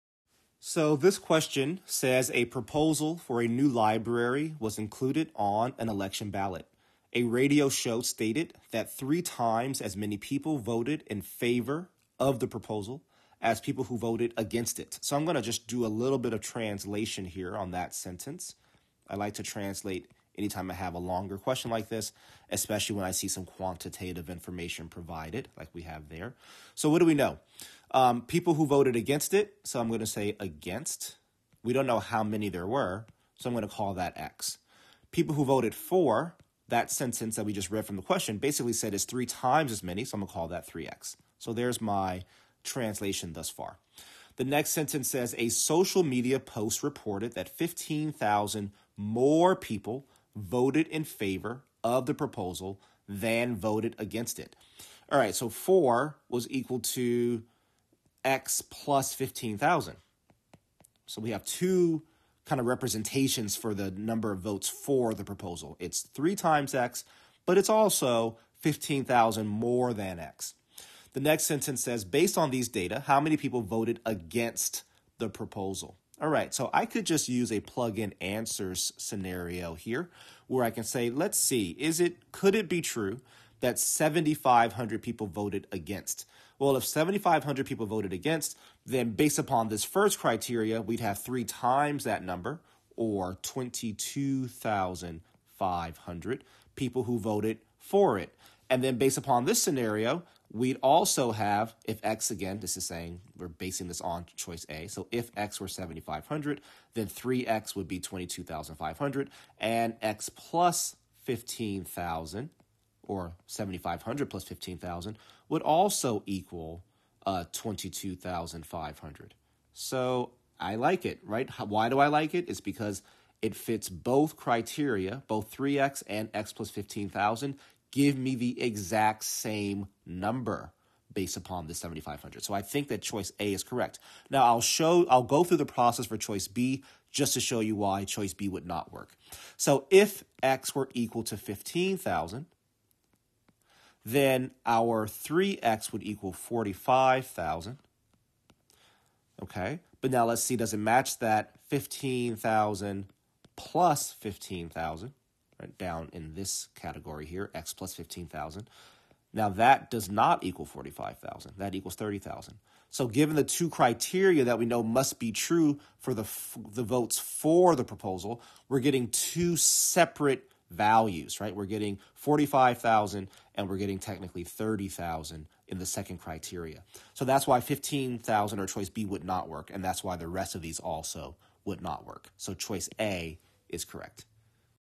A proposal for a new library was included on an election ballot. A radio show stated that 3 times...
Bluebook Digital SAT Test 2 Module 2 (Hard) Question 4:

A proposal for a new library was included on an election ballot. A radio show stated that 3 times as many people voted in favor of the proposal as people who voted against it. A social media post reported that 15,000 more people voted in favor of the proposal than voted against it. Based on these data, how many people voted against the proposal?